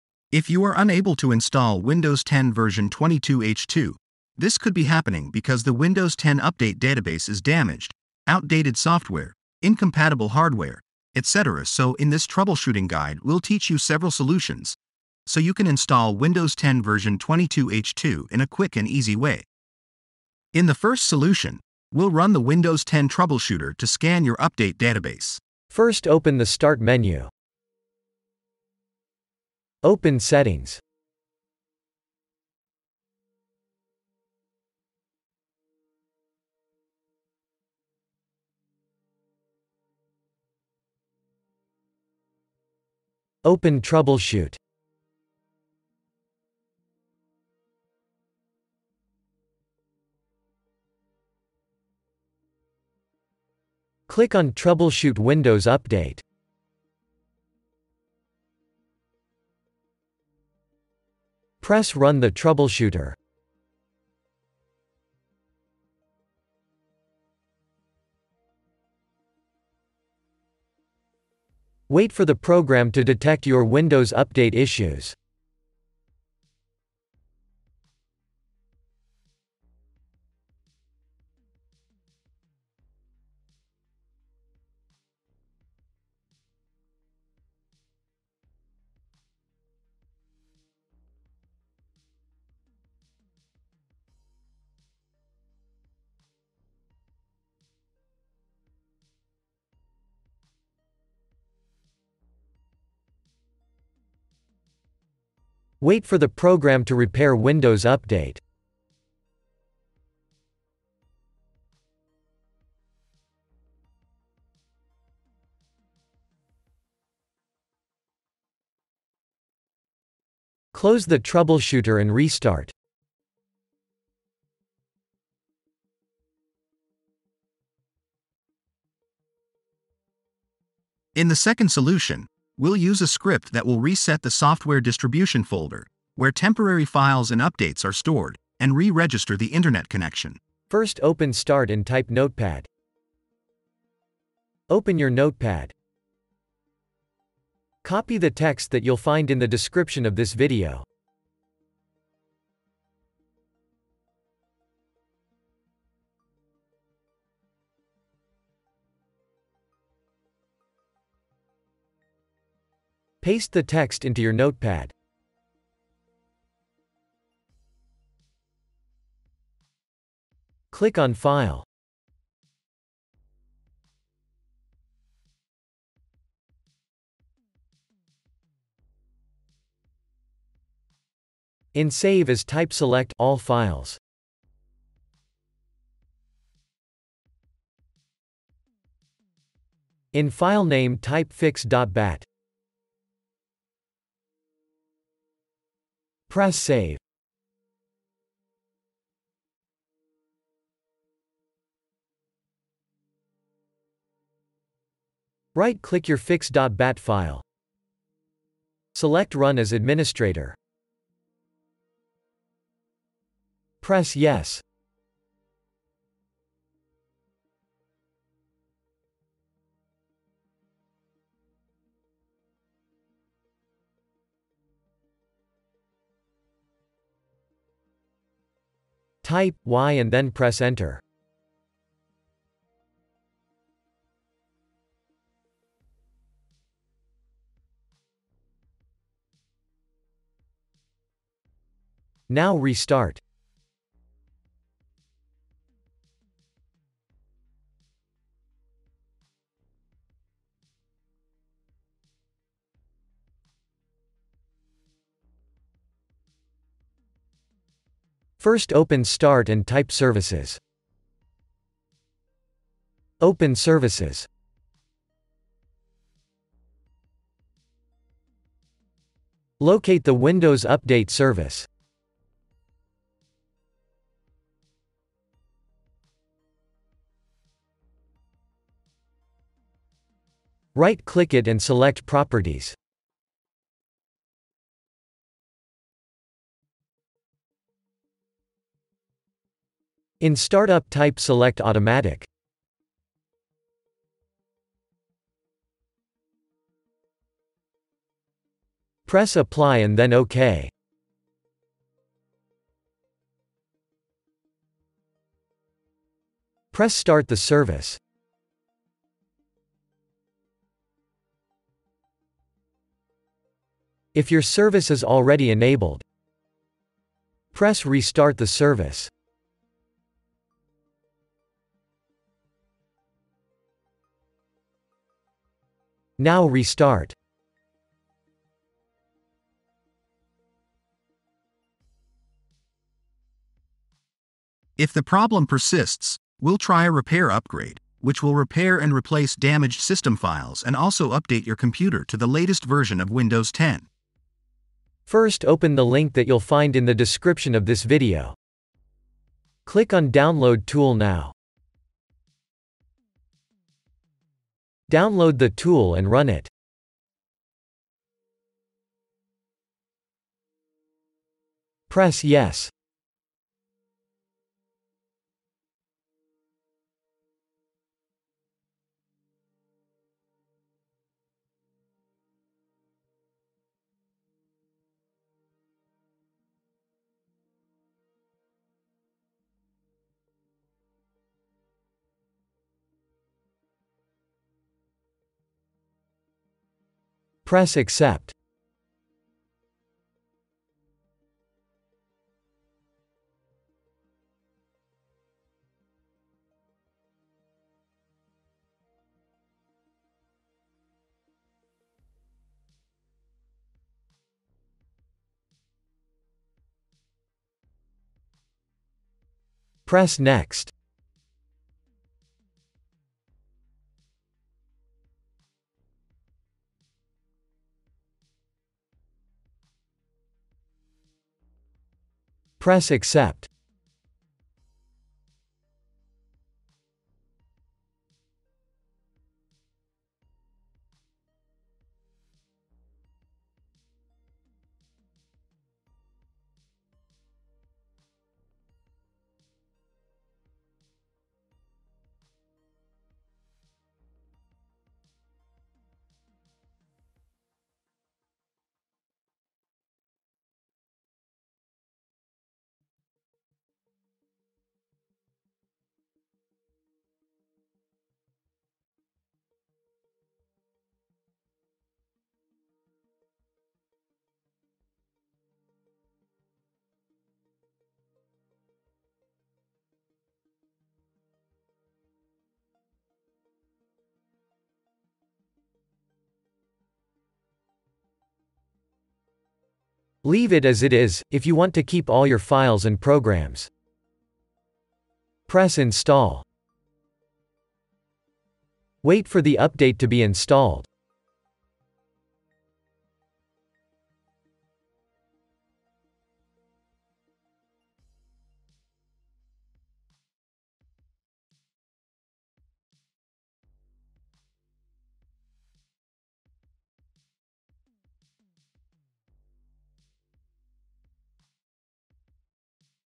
Windows 10 Update 22H2 Fails to Install FIX - [Tutorial]
If you are unable to install Windows 10 version 22H2, this could be happening because the Windows 10 update database is damaged, outdated software, incompatible hardware, etc. so in this troubleshooting guide we'll teach you several solutions, so you can install Windows 10 version 22H2 in a quick and easy way.

How to Repair Windows 10 Update Version 22H2 Failed to Install Error. Solved: Fail to Install/Update windows 10 version 22H2.